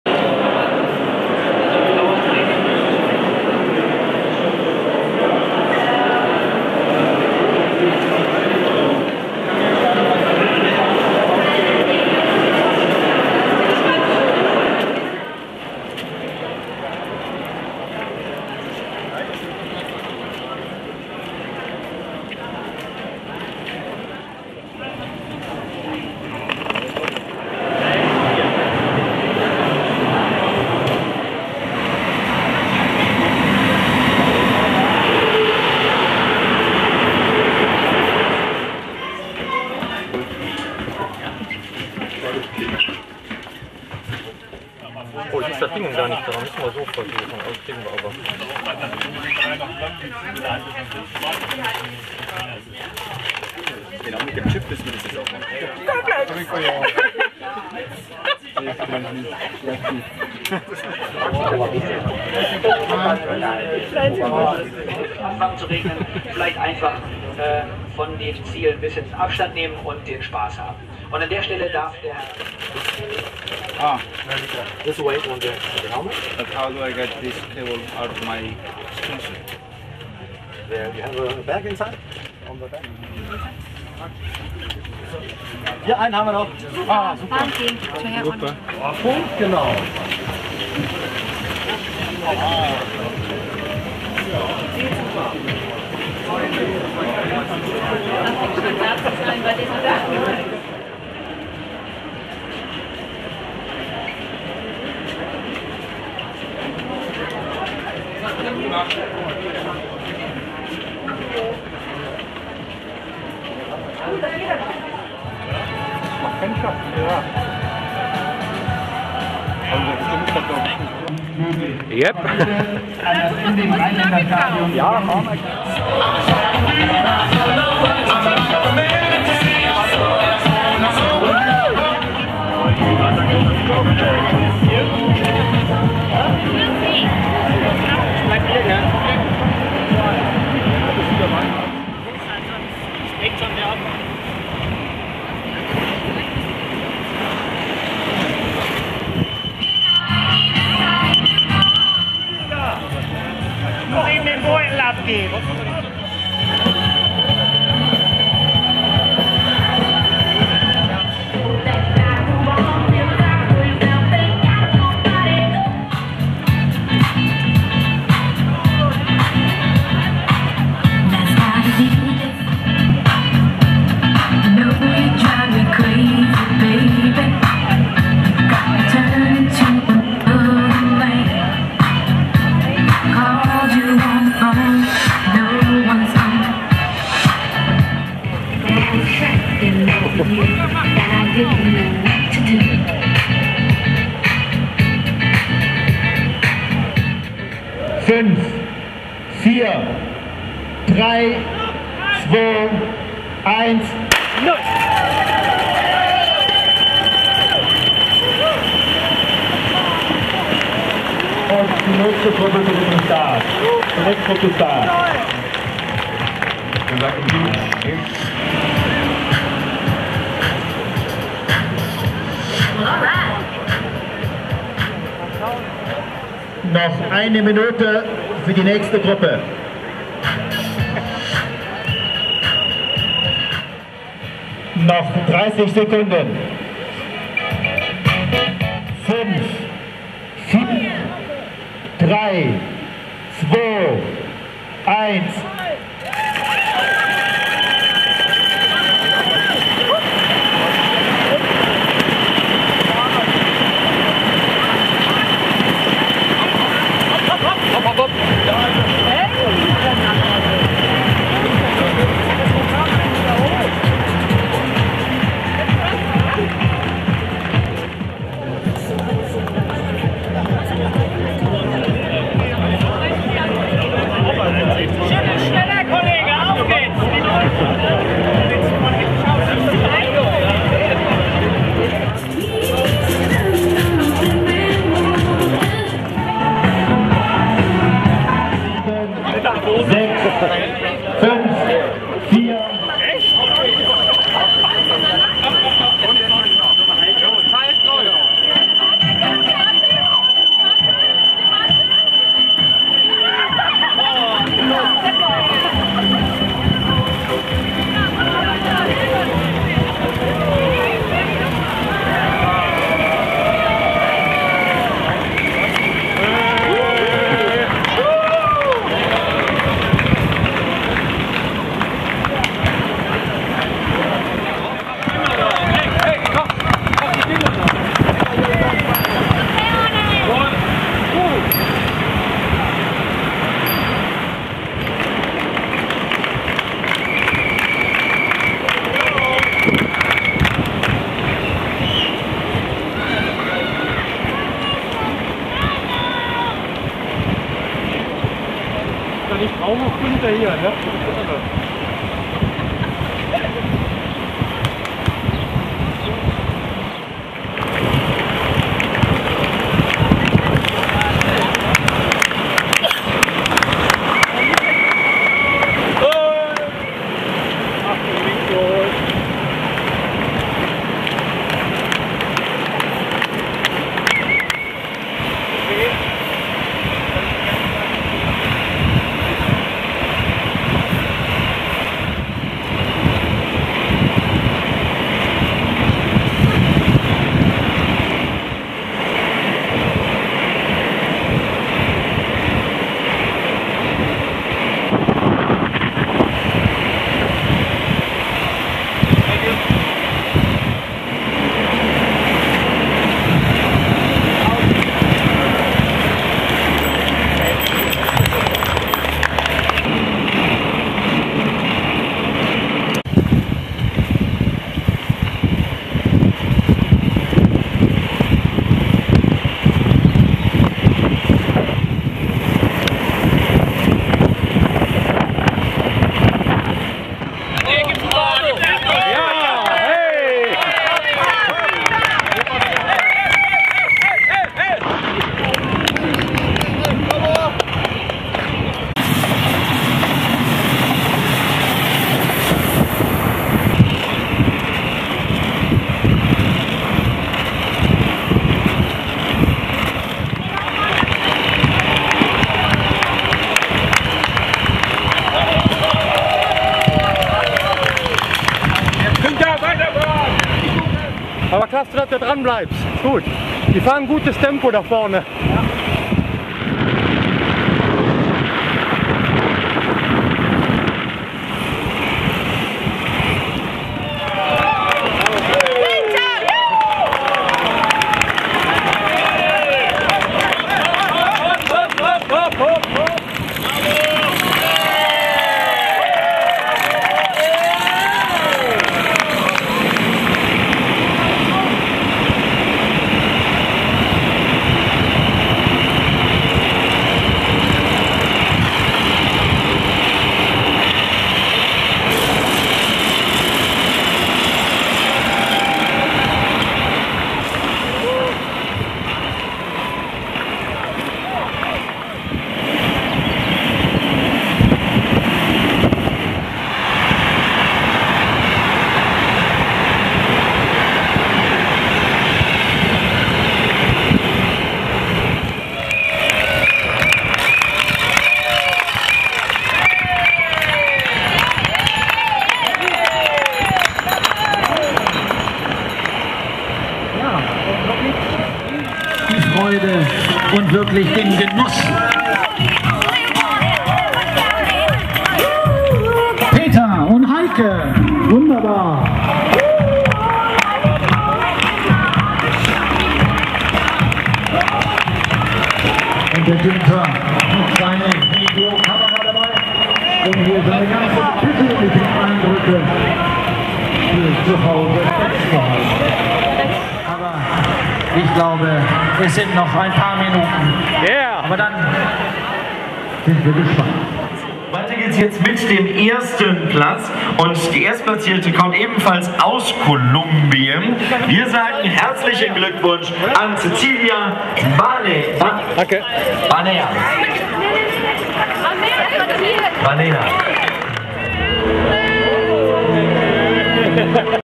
4. Mittelrhein Inline Marathon 2008
Von Oberwesel nach Koblenz. Im Video fehlt die Kopfsteinpflasterpassage in Rhens, da dort gerade der Akku der Kamera streikte. Im Sonderzug auf dem Weg zum Start nach Oberwesel war die Weltelite vertreten, im Bild sind kurz Luca Saggiorato und Diego Rosero zu sehen.

Tags: Speedskating Skating Rollerblade German Inline Cup Marathon Boppard in-line skeeler challenge inliner marathon Roller